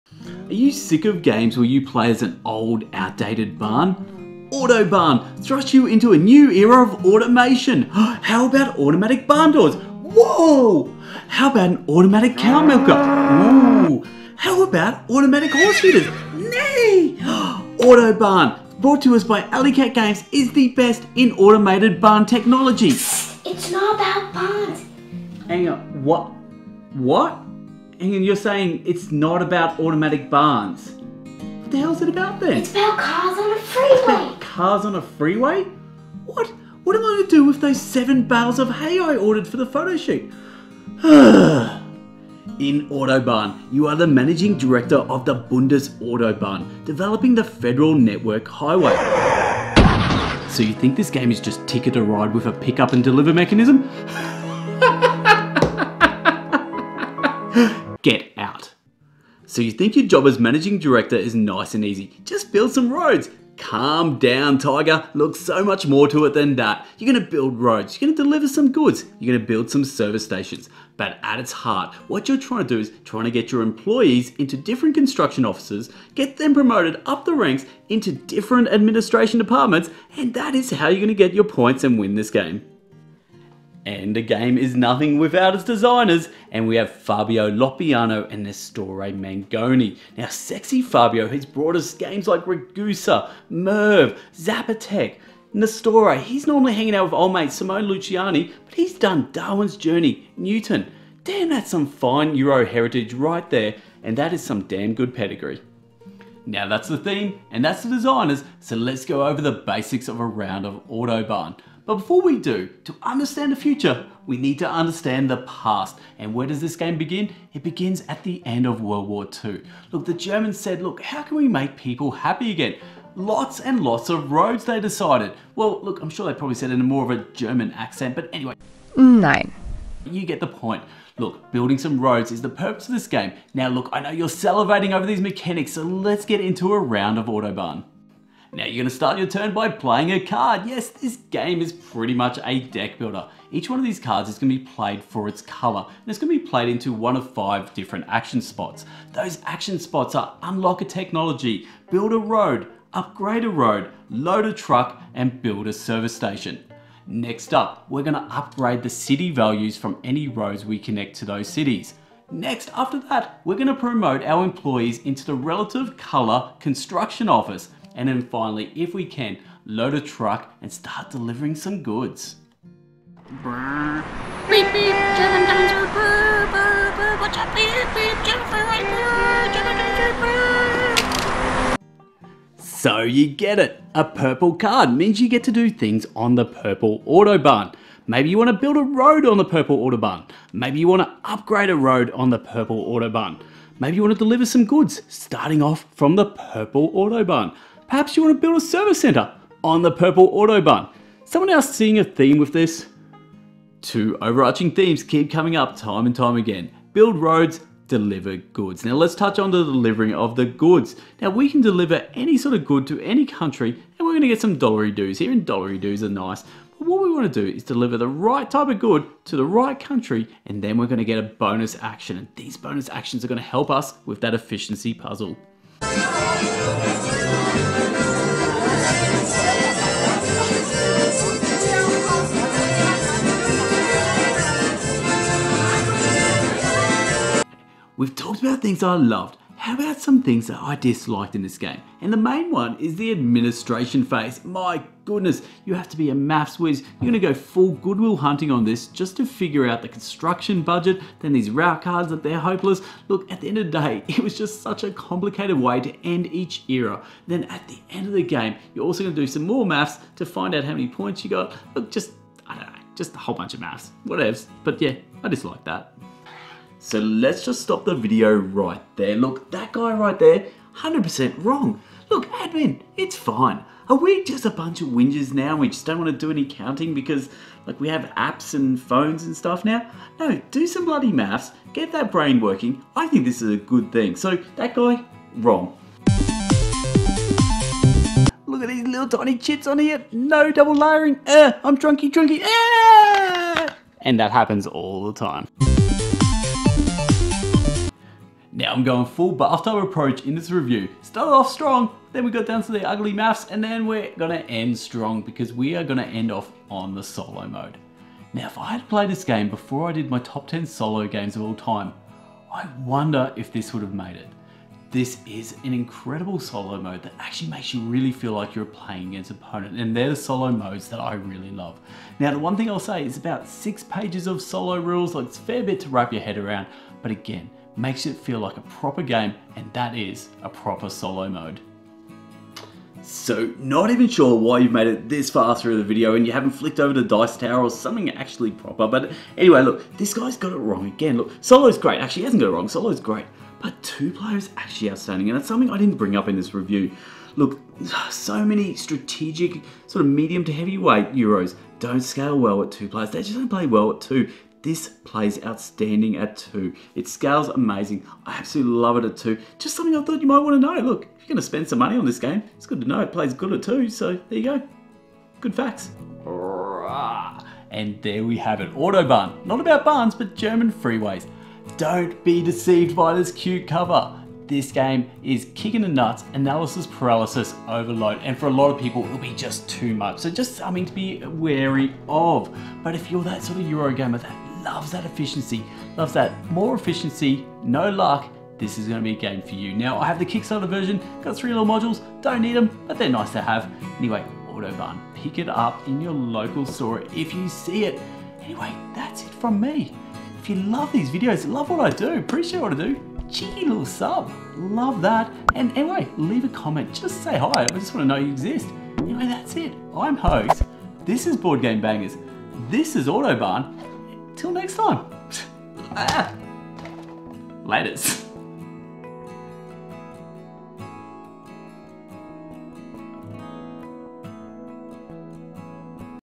Autobahn Review: Why This Driver's Paradise Continues to Amaze
In this Autobahn board game review, we take a look at the latest German game from designer Dirk Henn. Autobahn is a fast-paced game of highway racing that unfortunately has nothing to do with automated barns!

If you're a fan of German board games, be sure to check out Autobahn. This game is a lot of fun and definitely worth playing. While it doesn't have anything to do with automated barns, it's still an entertaining game that you'll love.